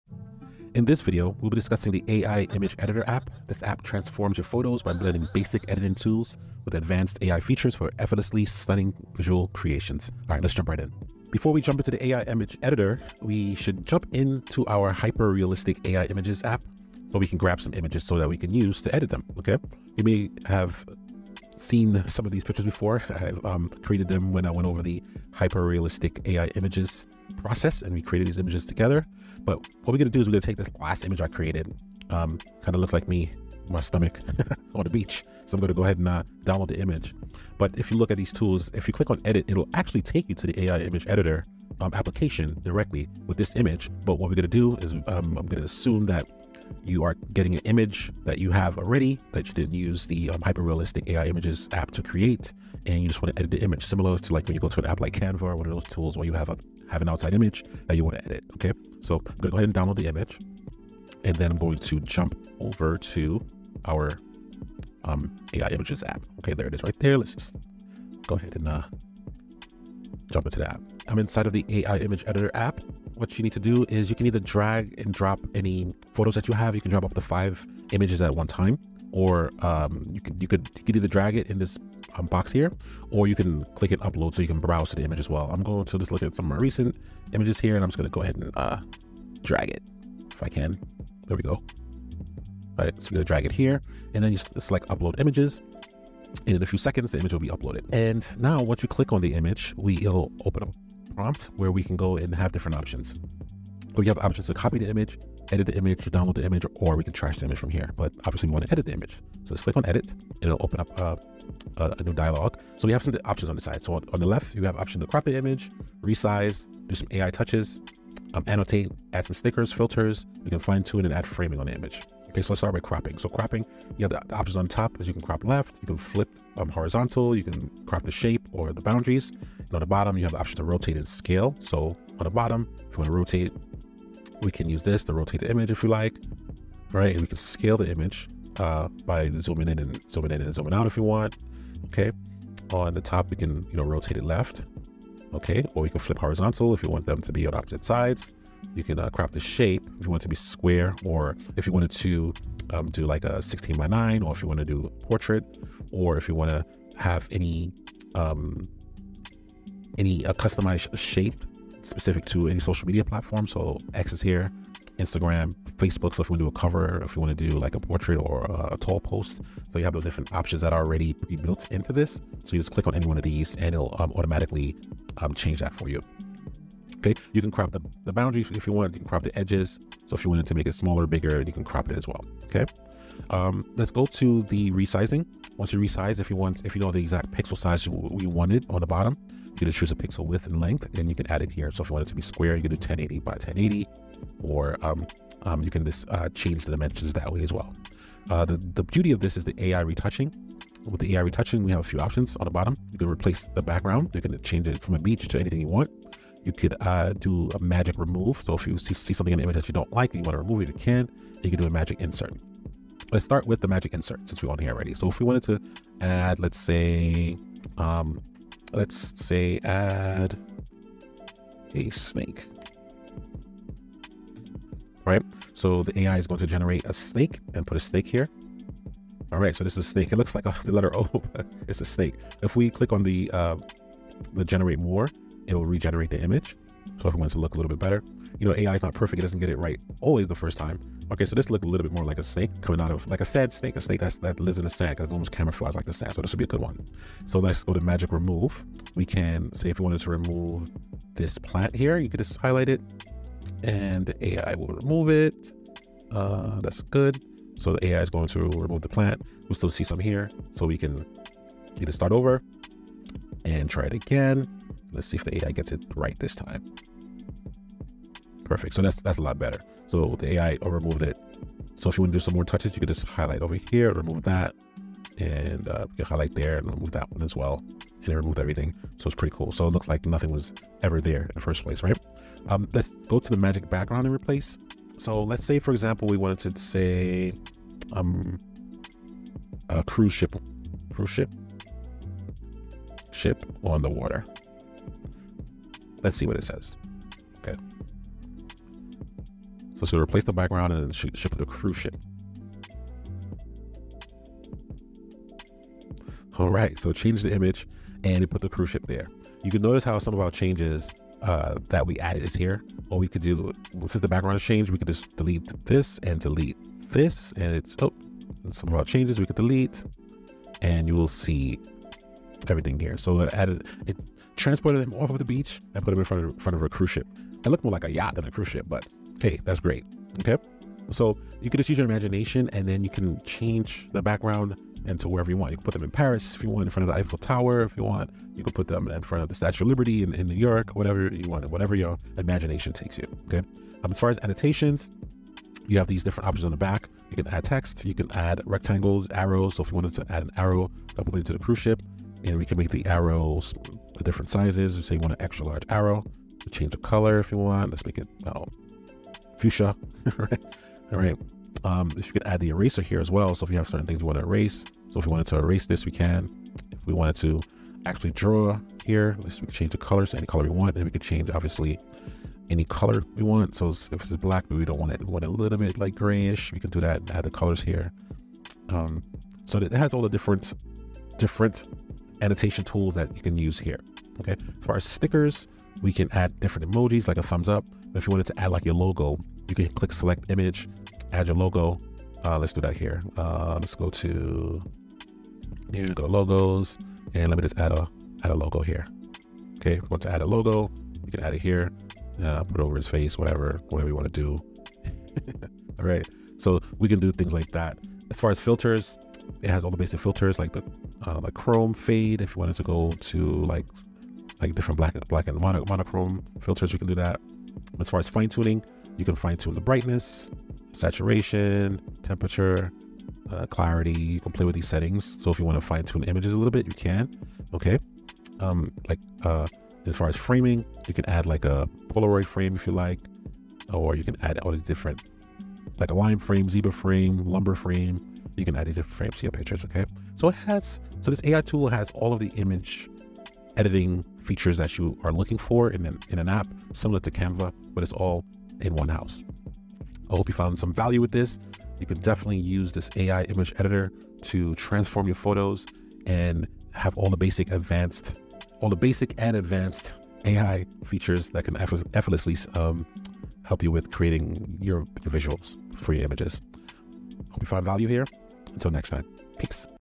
Unleash Your Creativity with Our AI Image Editor! 🎨
Looking to elevate your visual content without the hassle? 🚀

 Introducing our AI Image Editor, the ultimate tool designed for marketers and creatives to effortlessly edit and enhance images. In this video, we'll showcase how intelligent AI tools can automate tedious tasks, allowing you to bring your visuals to life in just a few clicks.

Discover how our AI Image Editor helps you:
- Save Time: Automate complex editing processes instantly.
- Enhance Quality: Improve image resolution and aesthetics effortlessly.
- Add Creative Flair: Apply filters, effects, and adjustments tailored to your brand.
- User-Friendly Interface: Designed for ease of use—no design skills required!

Whether you're crafting social media posts, marketing materials, or website graphics, our AI Image Editor empowers you to produce professional-quality visuals with minimal effort. Say goodbye to complicated software and hello to seamless creativity! 🌟